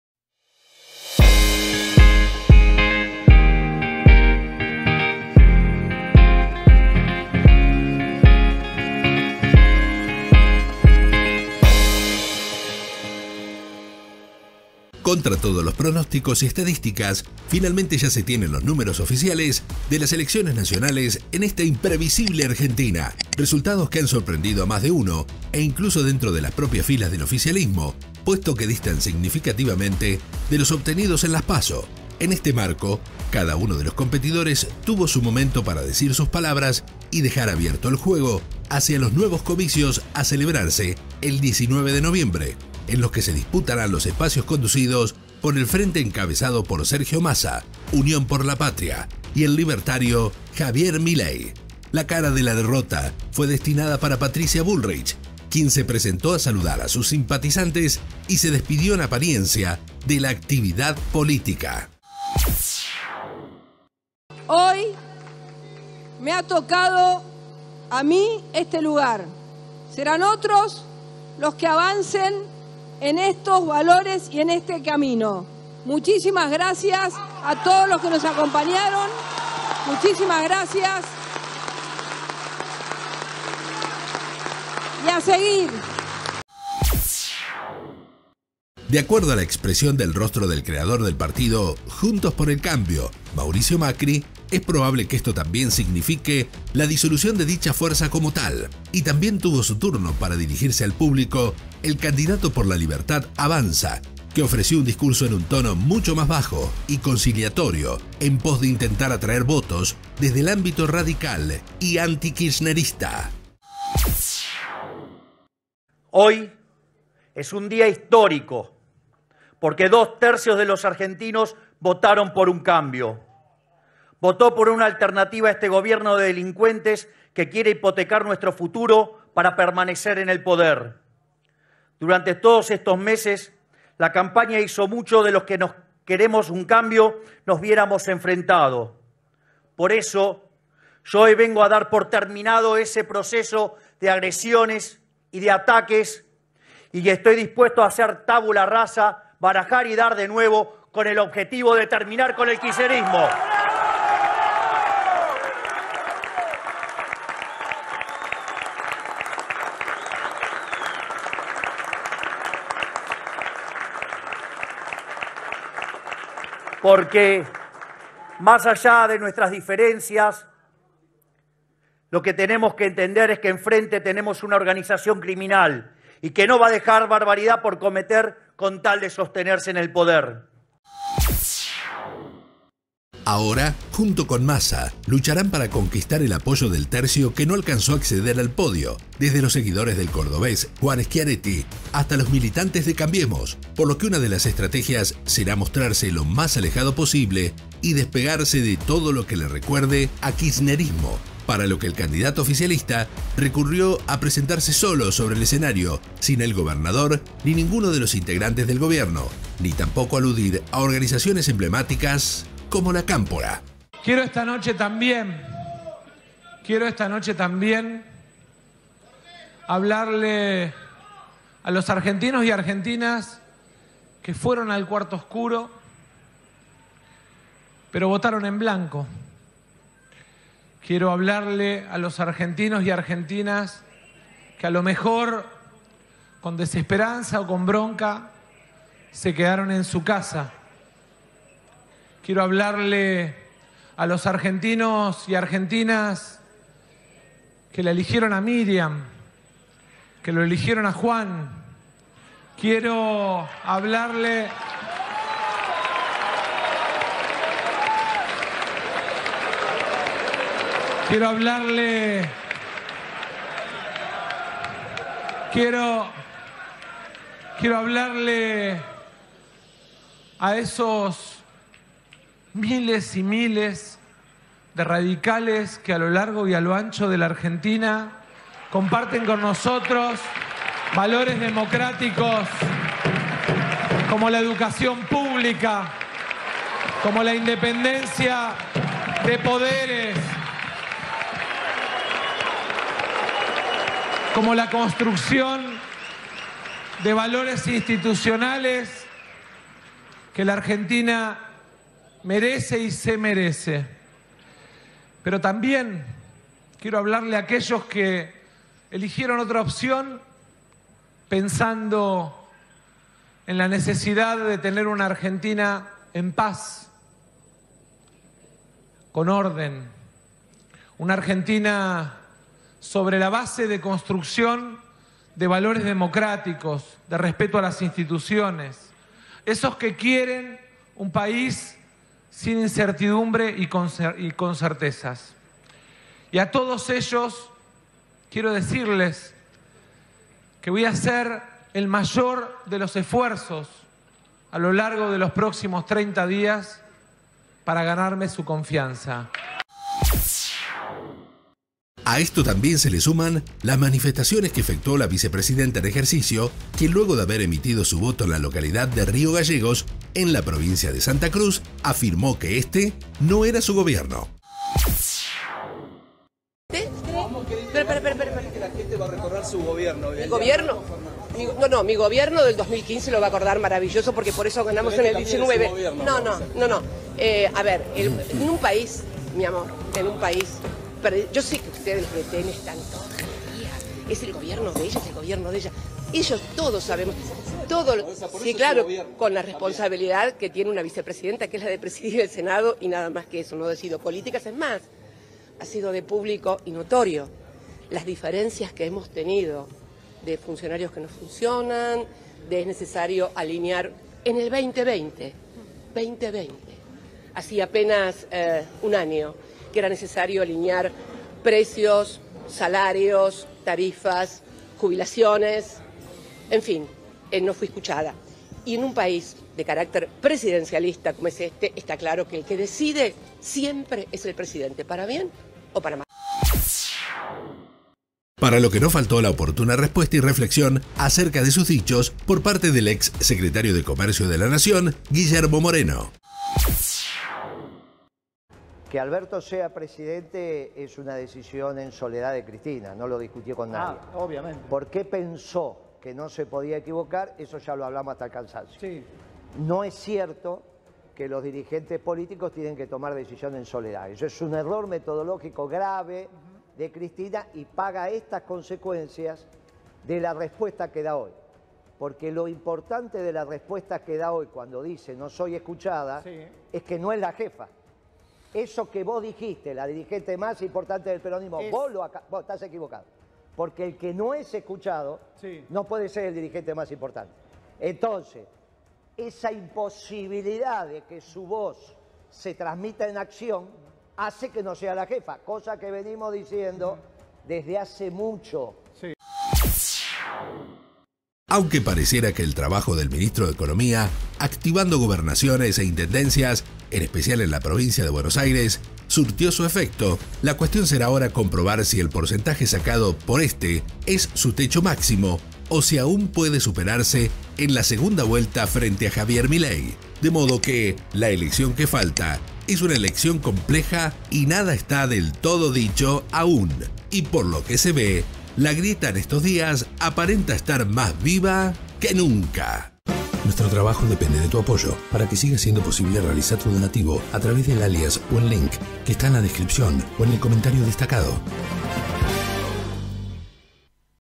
HAY BALOTAJE 🗳️ CISNE NEGRO 🇦🇷 (#49 INFORME)
Contra todos los pronósticos y estadísticas, finalmente ya se tienen los números oficiales de la elecciones nacionales en esta imprevisible Argentina
Resultados que ha sorprendido a más de uno e incluso dentro de las propias filas del oficialismo, puesto que distan significativamente de los obtenidos en las PASO
En este marco, cada uno de los competidores tuvo su momento para decir sus palabras y dejar abierto el juego hacia los nuevos comicios a celebrarse el 19 de noviembre, en los que se disputarán los espacios conducidos por el frente encabezado por Sergio Massa, Unión por la Patria y el libertario, Javier Milei
La cara de la derrota fue destinada para Patricia Bullrich, quien se presentó a saludar a sus simpatizantes y se despidió en apariencia, de la actividad política

De acuerdo a la expresión del rostro del creador del partido "Juntos por el Cambio" Mauricio Macri, es probable que ésto también signifique la disolución de dicha fuerza como tal
Y también tuvo su turno para dirigirse al público el candidato por La Libertad Avanza, que ofreció un discurso en un tono mucho más bajo y conciliatorio en pos de intentar atraer votos desde el ámbito radical y "antikirchnerista"